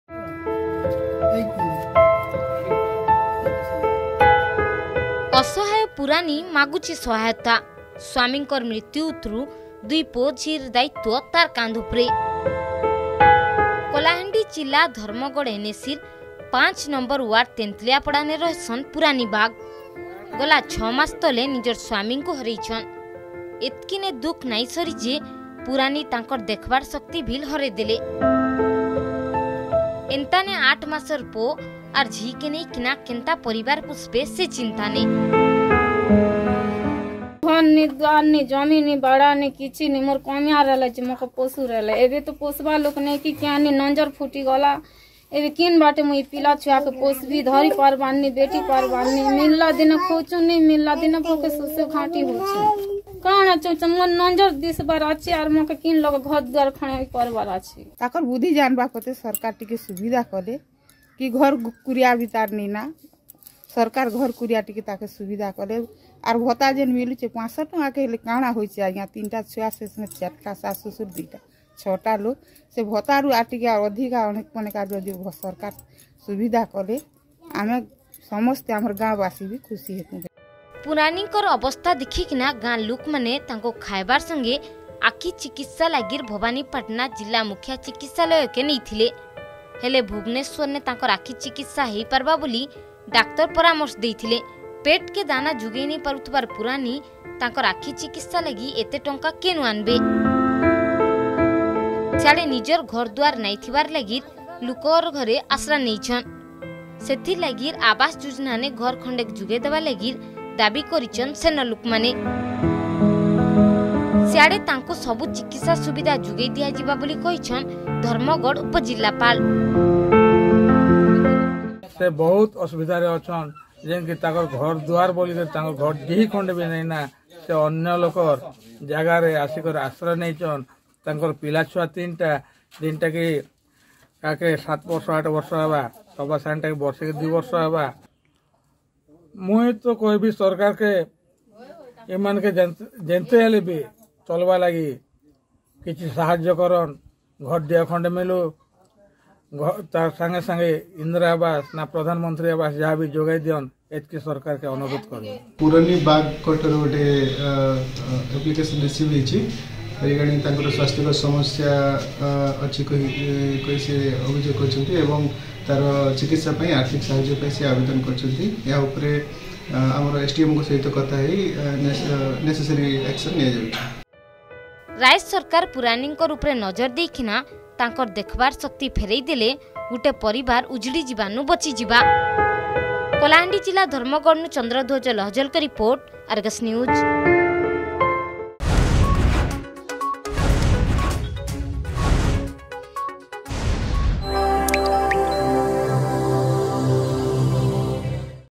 ଅସହାୟ ପୁରାଣି ମାଗୁଛେ ସହାୟତା
ଅସହାୟ ପୁରାଣି ମାଗୁଛେ ସହାୟତା । ସ୍ବାମୀଙ୍କର୍ ମୃତ୍ୟୁ ଉତରୁ ଦୁଇ ପୋଅ, ଝି ର୍ ଦାୟିତ୍ବ ତାର୍ କାନ୍ଧ ଉପରେ । କଳାହାଣ୍ଡି ଜିଲ୍ଲା ଧର୍ମଗଡ ଏନଏସିର ୫ନଂ ୱାର୍ଡ ତେନ୍ତୁଲିଆପଡାନେ ରହେସନ୍ ପୁରାଣୀ ବାଗ । ଗଲା ୬ ମାସ ତଲେ ନିଜର୍ ସ୍ବାମୀଙ୍କୁ ହରେଇଛନ୍ । ଏତକି ନେ ଦୁଃଖ୍ ନାଇ ସରିଯେ ପୁରାଣୀ ତାକଁର୍ ଦେଖବାର୍ ଶକ୍ତି ଭିଲ୍ ହରେଇଦେଲେ ।